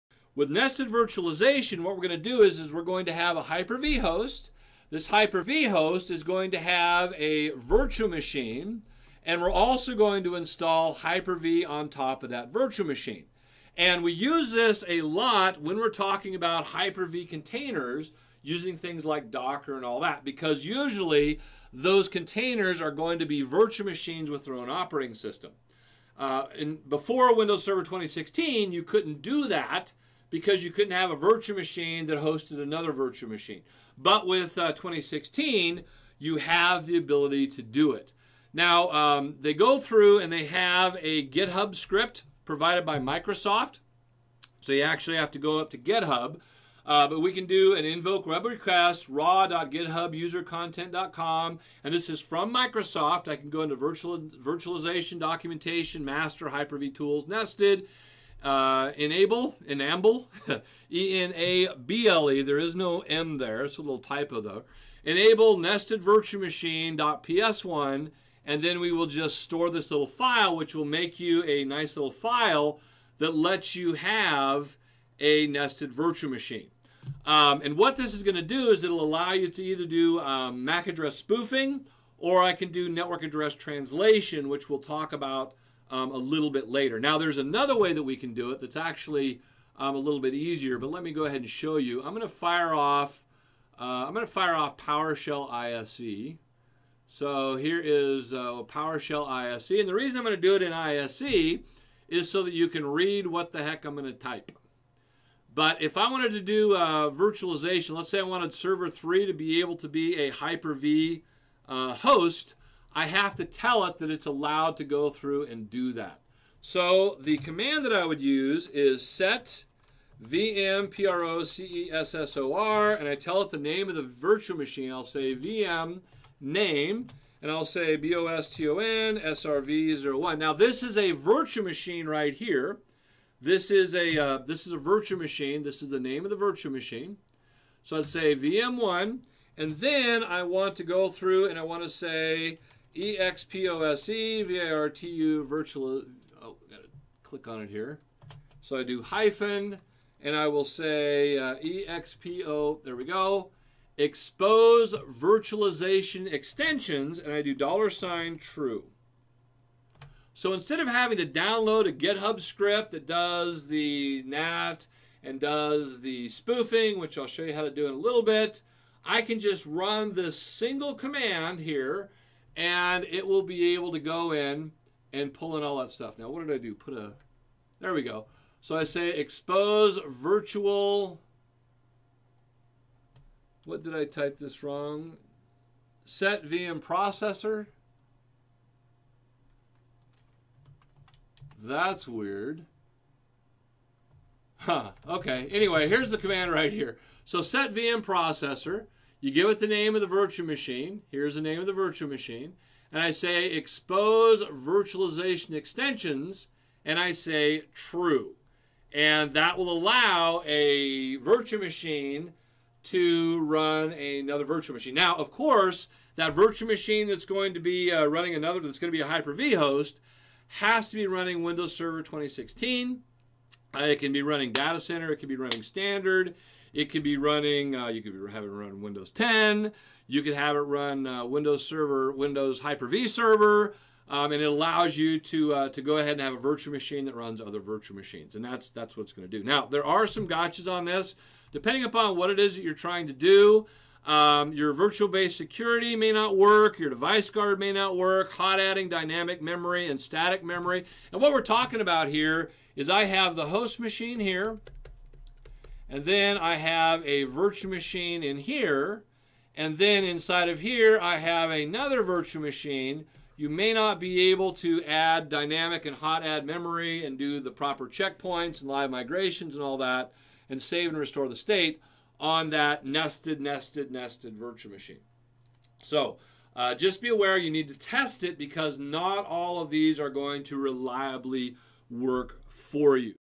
Nested Virtualization in Hyper-V
It used to be super difficult to do nested virtualization. Now, it is quite a bit easier. Doug Bassett, Stormwind instructor discusses nested virtualization, a pretty nifty feature in the newer implementations of Microsoft's virtualization product.